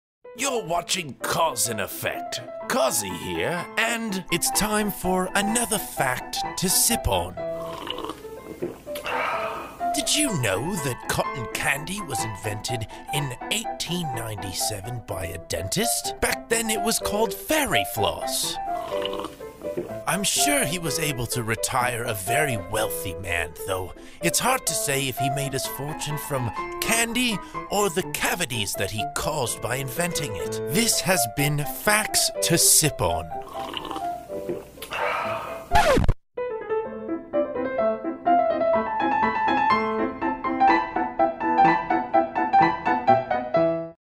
Cotton Candy Conspiracy - Facts To Sip On #1
Did you know that there is a Cotton Candy Conspiracy?
Learn the facts in the debut episode of "Facts To Sip On!"

Did you know that a dentist invented Cotton Candy in 1896?
Back then it was called fairy floss!
I’m sure he retired a very wealthy man,
though I'm not sure if he made his fortune from cleaning cavities
or selling the candy that caused it.